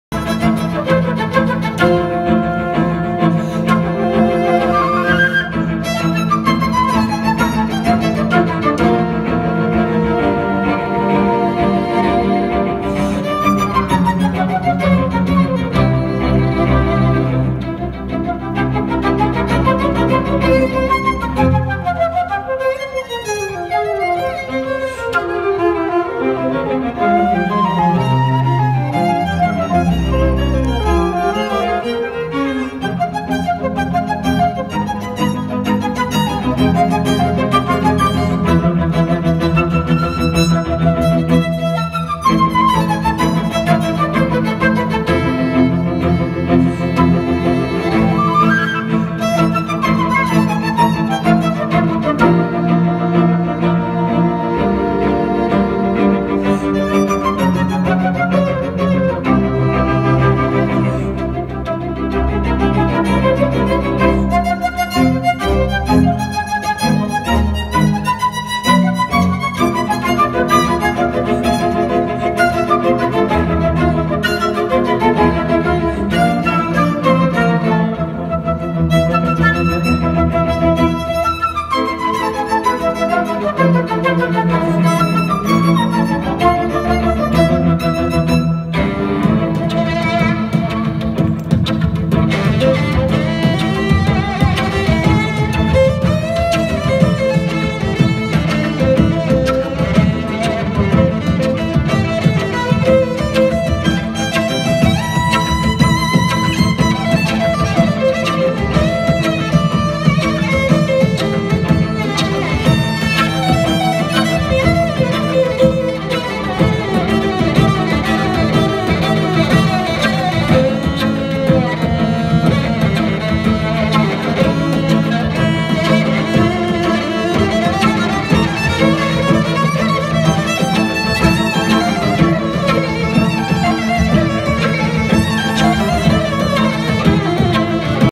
GNU Quartet a Roma Castel Sant’Angelo
Lo GNU Quartet a Roma Castel Sant’Angelo per artcity19. Paganini in chiave Rock è il loro cavallo di battaglia.